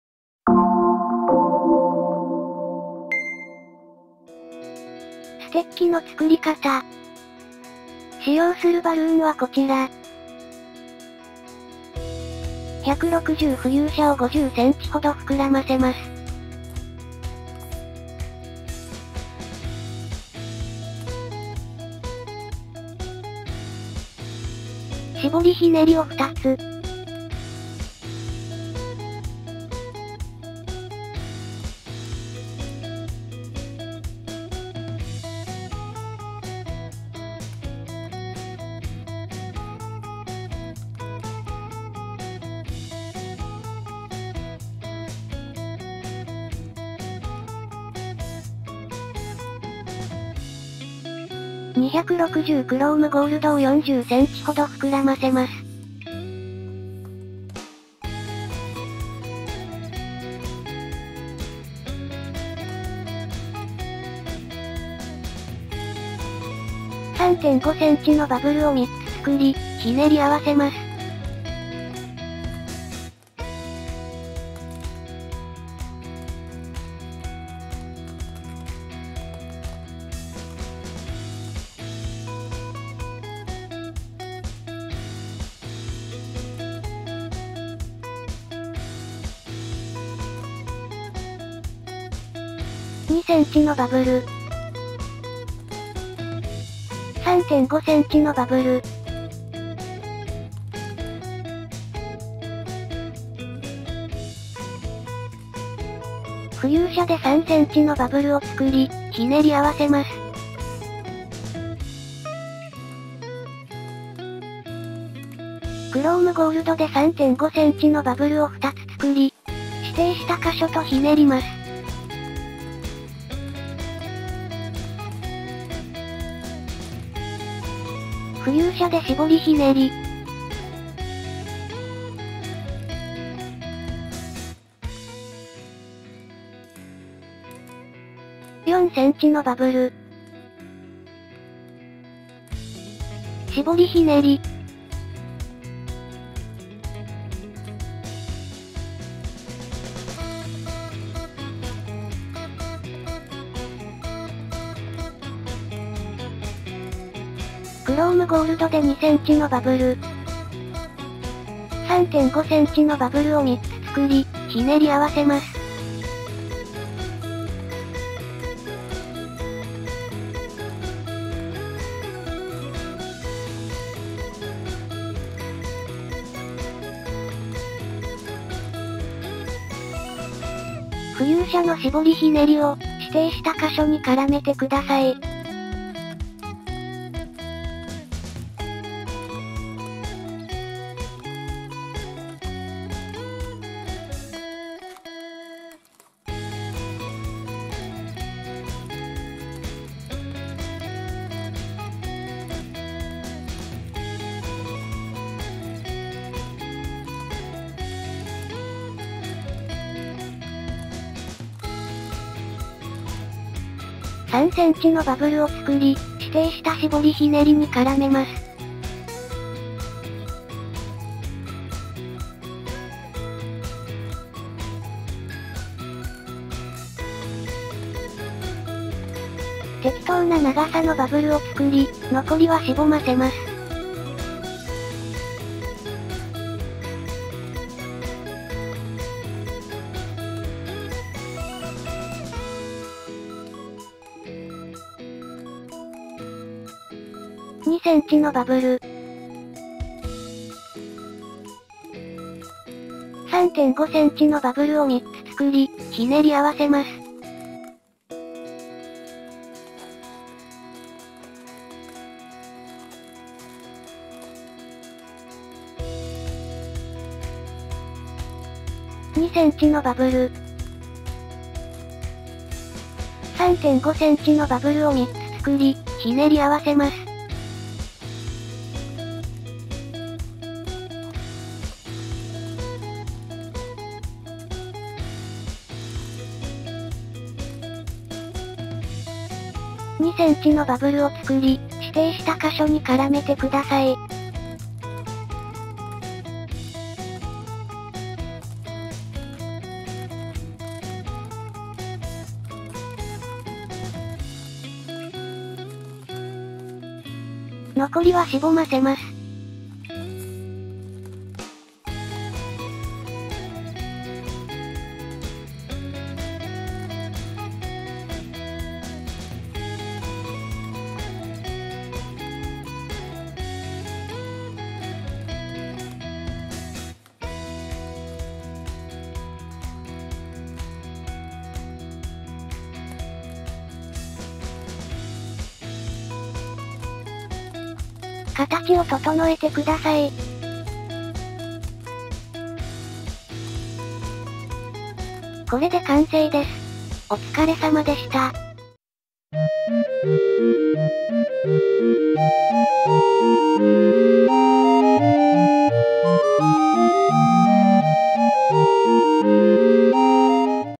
Magic Wand - How to Make Balloon Animals #124 / バルーンアートの作り方 #124 (ステッキ)
In this video, I will show you how to make a magic wand. Please use it as a balloon decoration or such!
バルーンアートで魔法の杖を作りましょう。飾りつけなどに是非！

We will be using the following balloons:
Chrome Gold - 260 size ... 1 Balloon
Fuchsia - 160 size ... 1 Balloon

You can watch this video with English subtitles. Please use the subtitle function on YouTube.

Upon Joining my YouTube membership, you can watch tutorial videos for members only. To become a member, please click the following link then follow the instructions on that screen.